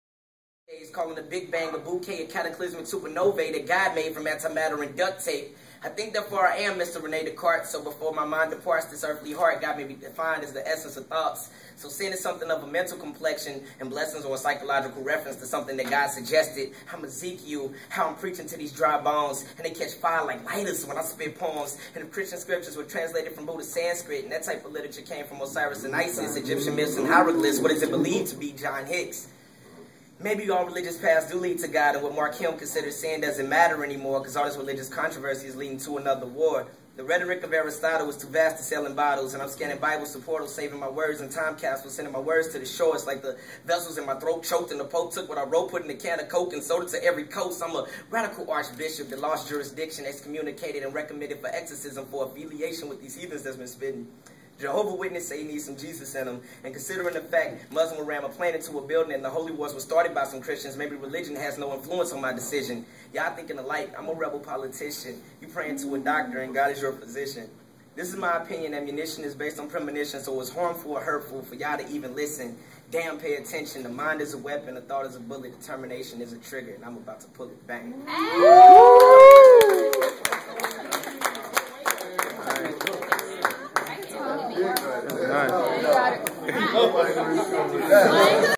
TONO and Common @ SLU with AAMS!!!
Student and African American Male Schoolar Tavon "TONO" Wilson rapping for Good Music Artist Common!!